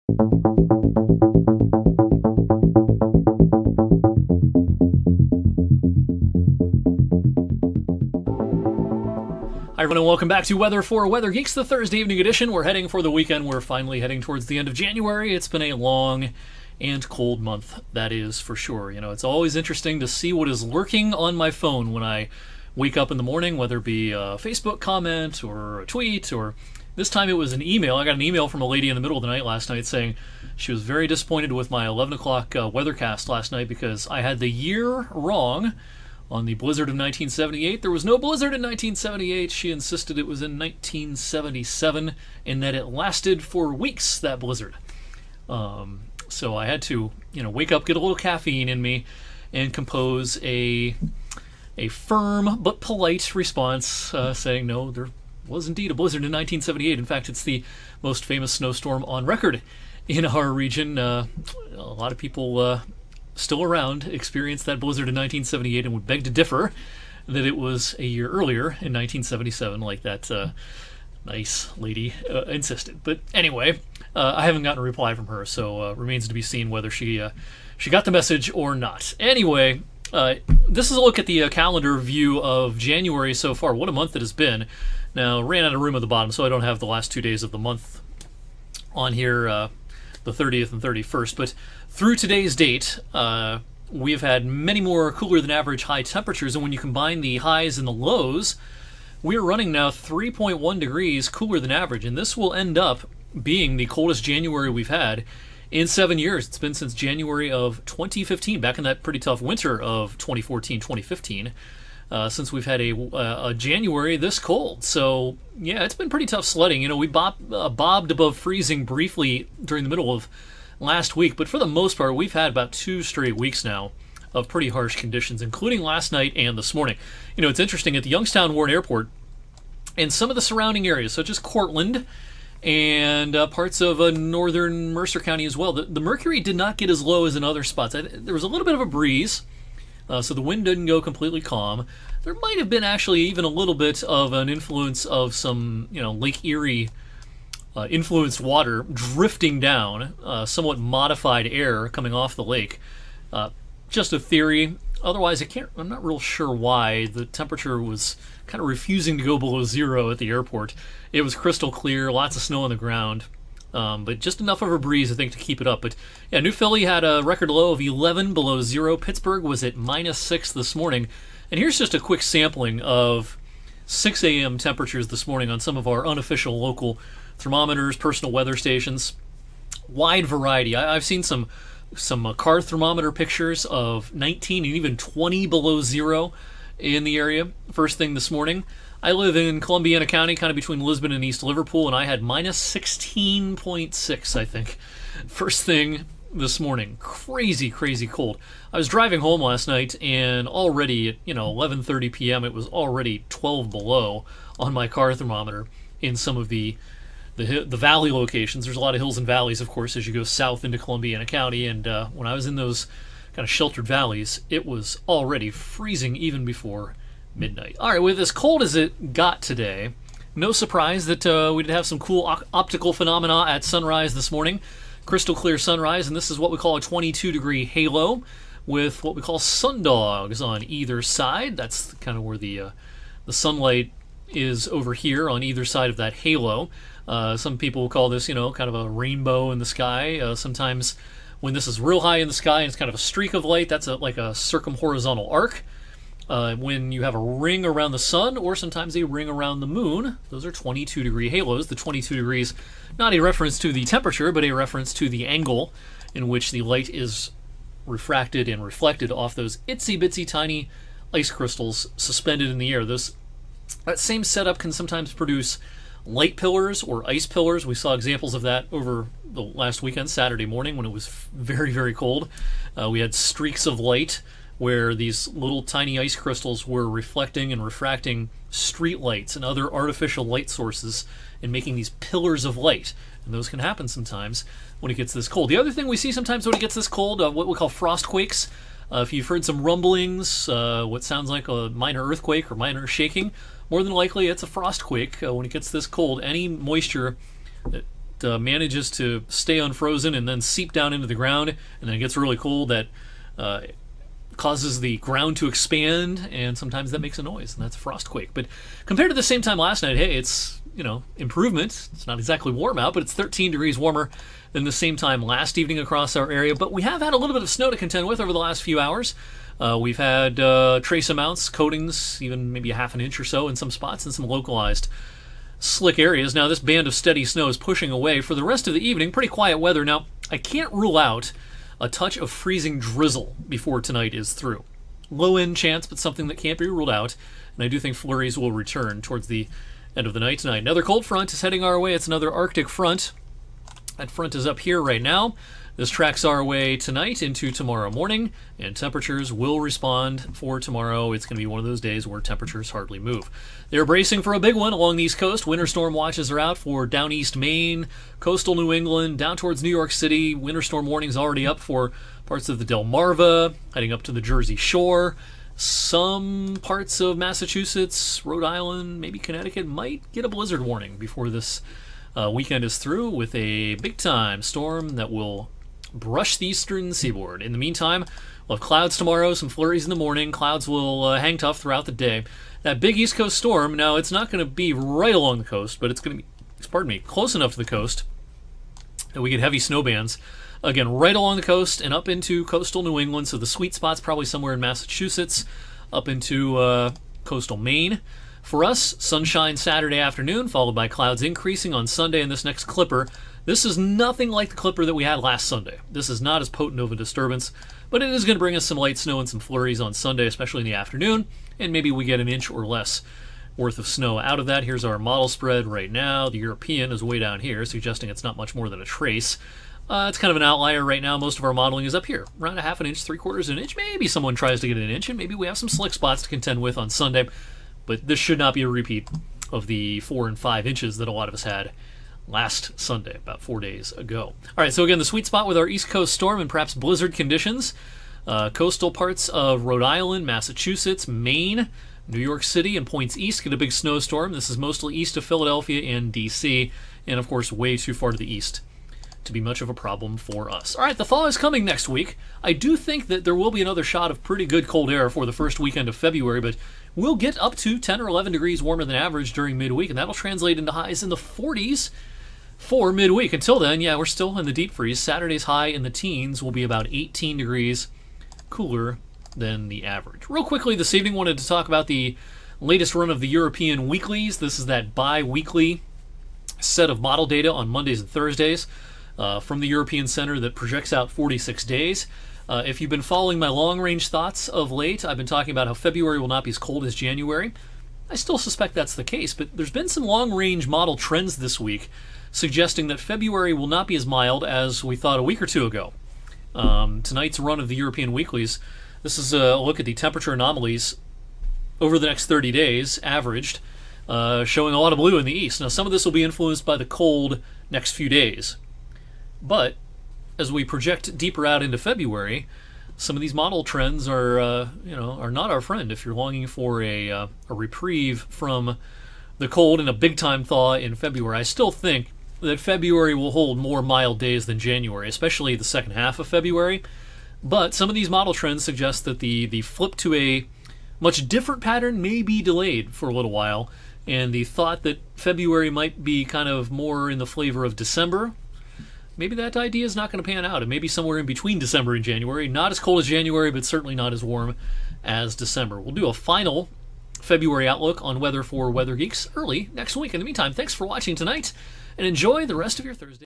After Coldest January In A While, How's February Looking? "Weather For Weather Geeks" 1/27/22
A respite from the cold is coming next week but the forecast is still a frigid one through the weekend. We'll see some light snow again before the weekend is through. February's outlook may be trending colder! This evening's video breaks down both the short and long range.